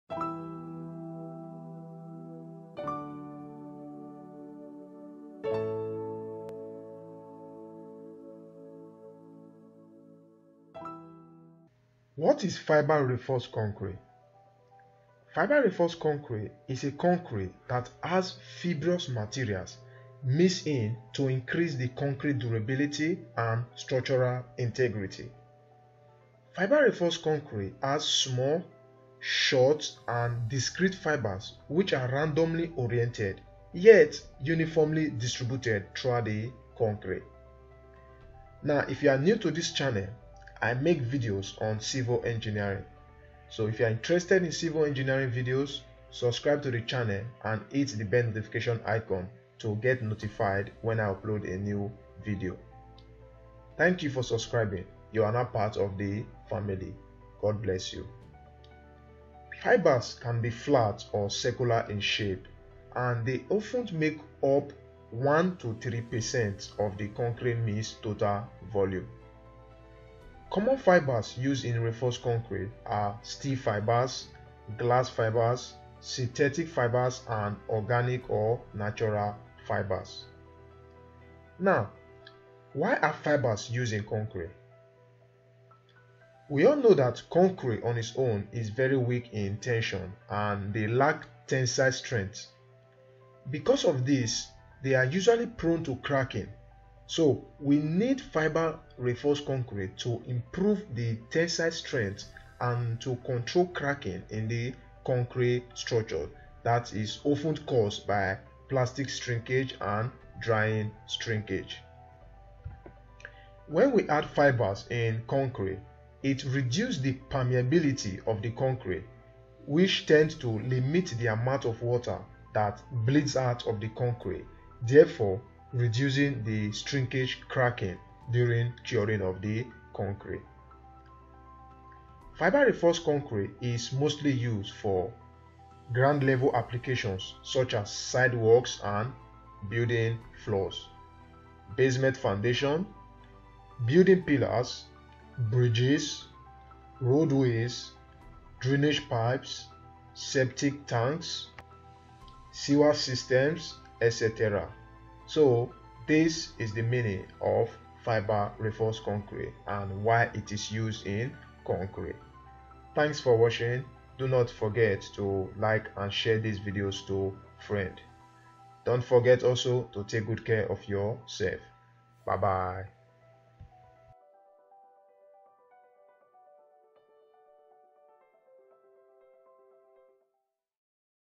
What is Fiber Reinforced Concrete?
Fiber reinforced concrete is concrete that has fibrous materials mixed in to increase the concrete durability and structural integrity.
Fiber reinforced concrete has small, short and discrete fibers which are randomly oriented yet uniformly distributed throughout the concrete.